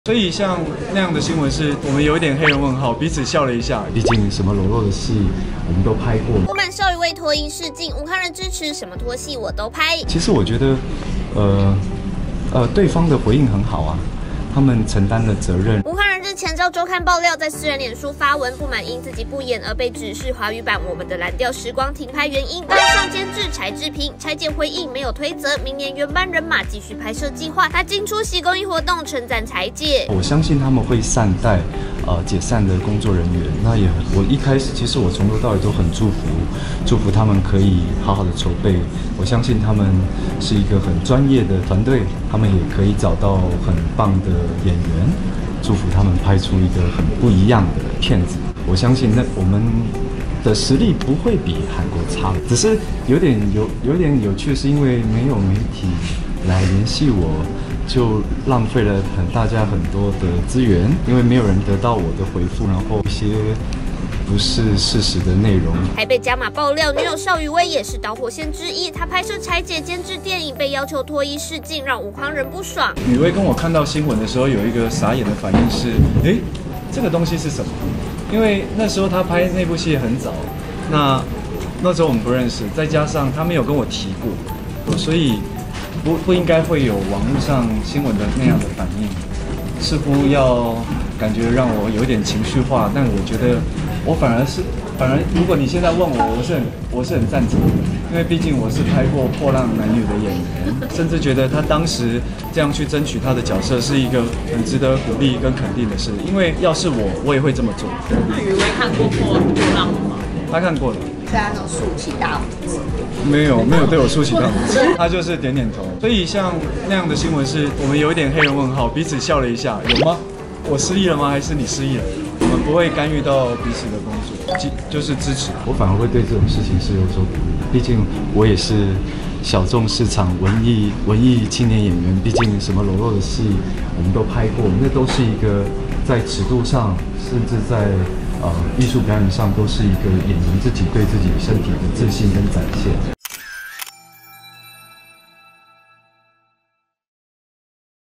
不滿 邵雨薇 脫衣試鏡？吳慷仁 支持「什麼脫戲我都拍」｜三立娛樂星聞
★訂閱【女孩要幹嘛】甜寵浪漫戲劇看到飽